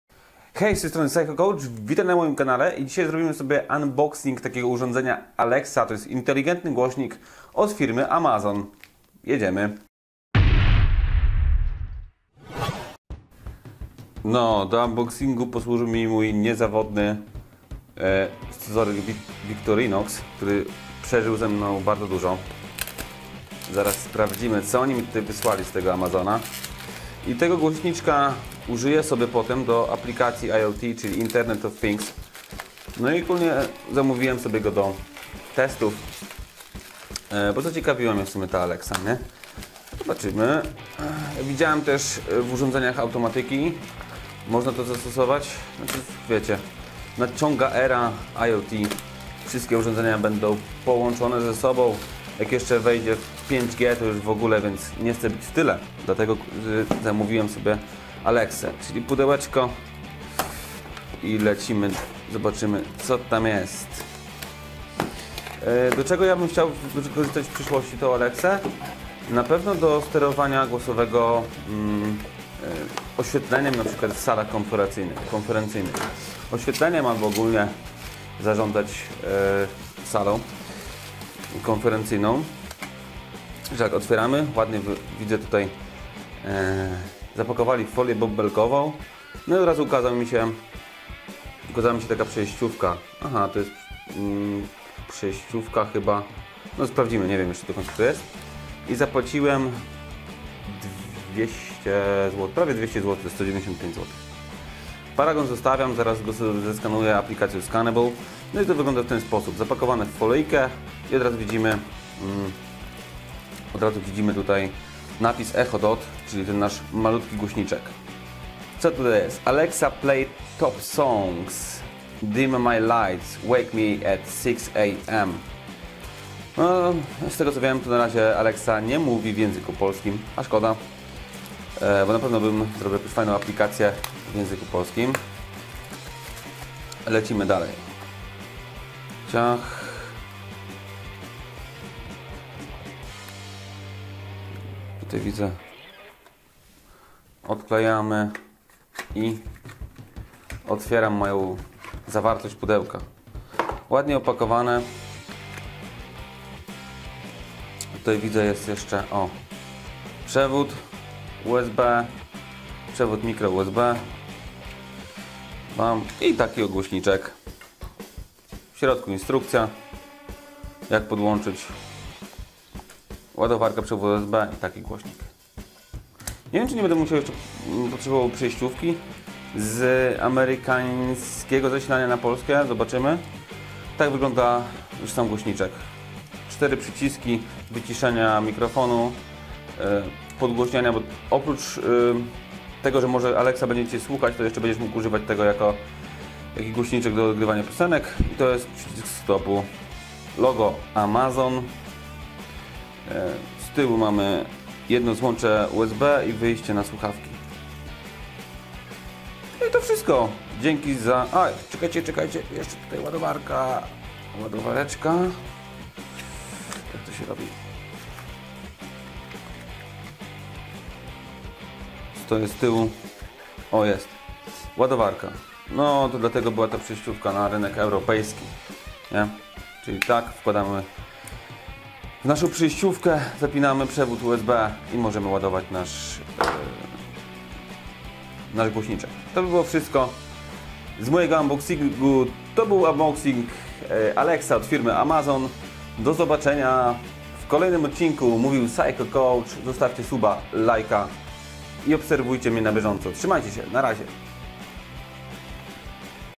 Szybki unboxing Alexy od Amazon!
Dzisiaj przyszedł do mnie inteligentny głośnik od Amazona - Alexa Dot. Rozpakowałem zobaczyłem i powoli będę go uruchamiał.